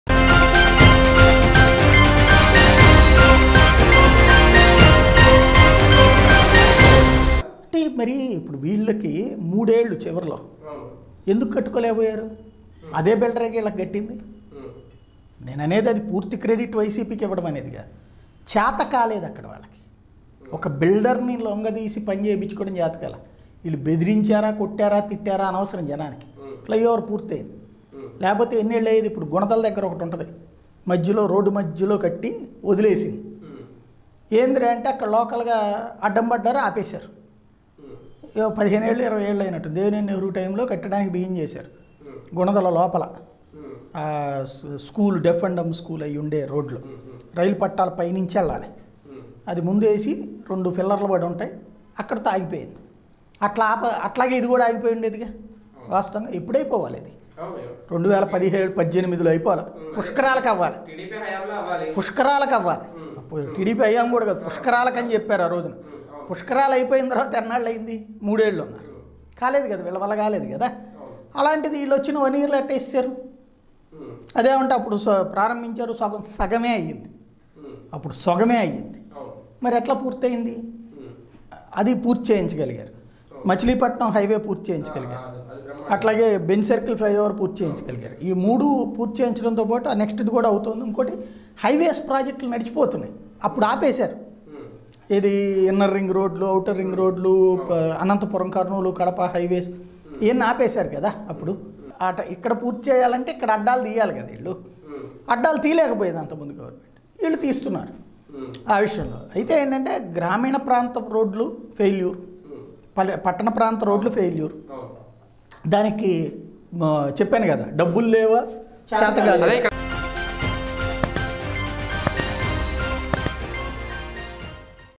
Journalist Sai | Jagan Ruling Is Different | ఎవరితో ఎలా పని చేపించాలి జగన్ కి తెలుసు | PDTV News
ఎవరితో ఎలా పని చేపించాలి జగన్ కి తెలుసు